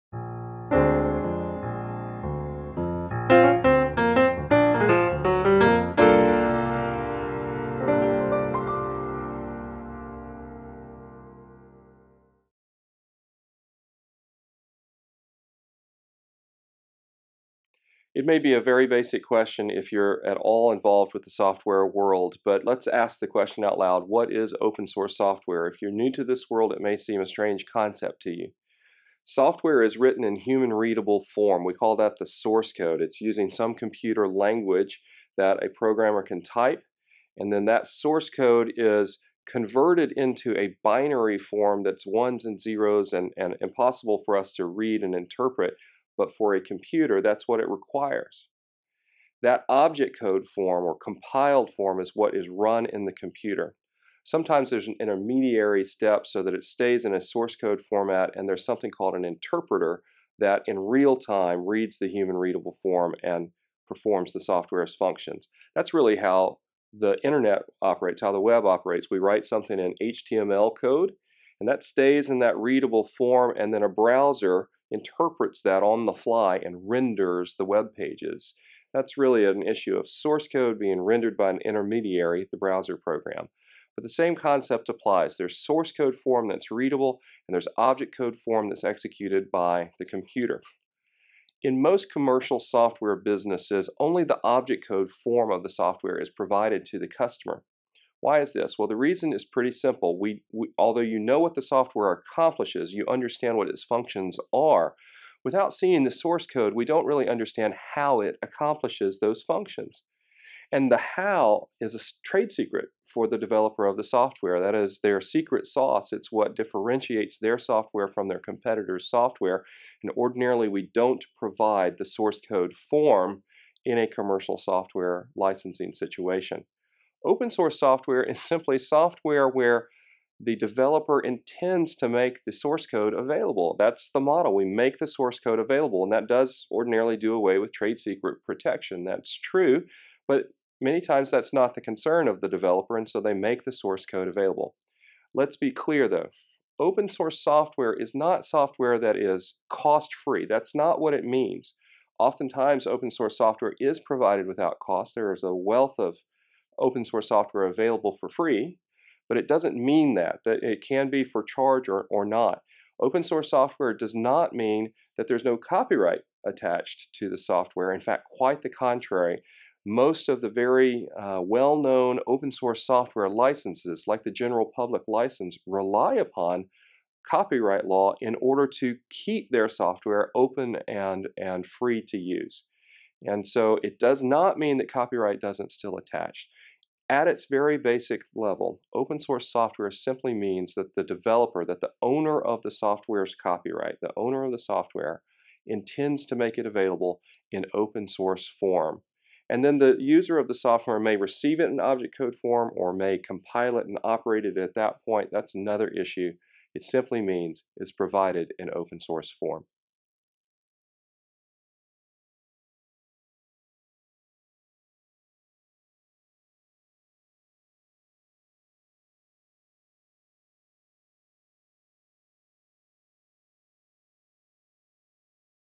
UpStart Workshop - Episode 23 - What is Open Source Software?
While most commercial software products are made available only in compiled, object code form, many proprietors of software make their software available in an open source model.  Open source software is simply software for which the source code is intentionally made available to licensees.  Copyrights still apply to open source software, and users of such software are subject to the software license accompanying (or negotiated for) the open source software, which may or may not permit modification, integration or distribution of the covered product.